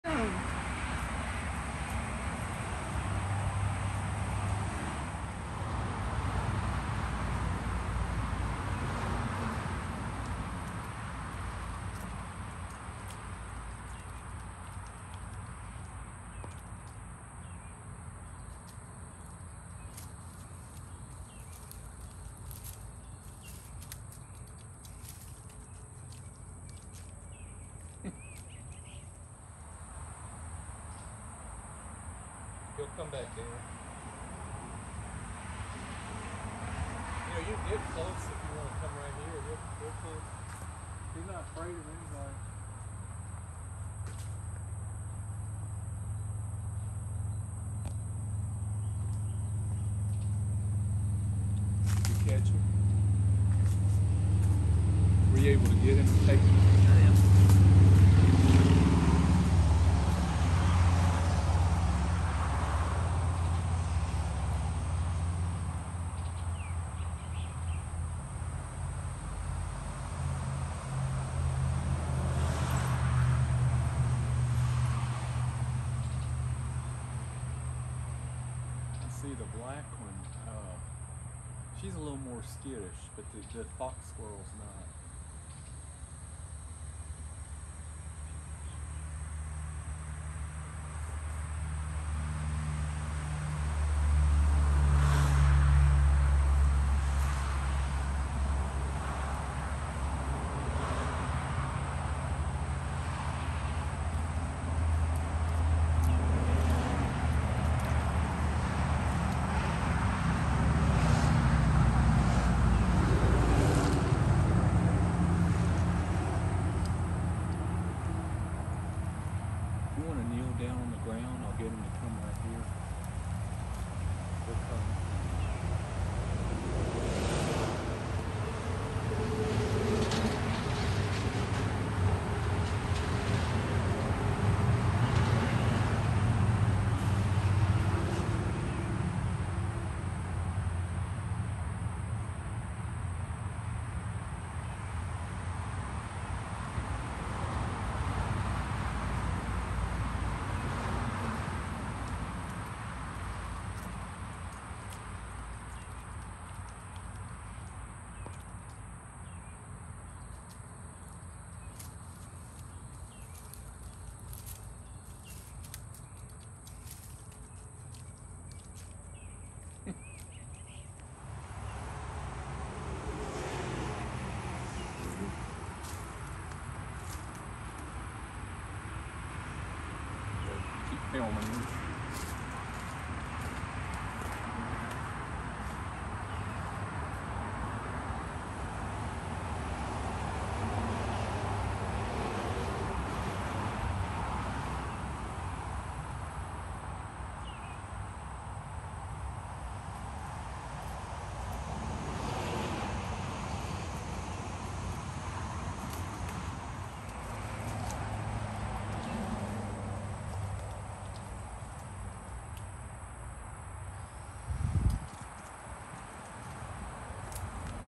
A Squirrel 🐿 Named Pecan Thief
The little fox squirrel loves pecan treats, but if you leave some in a container outside, he will take off with the whole thing.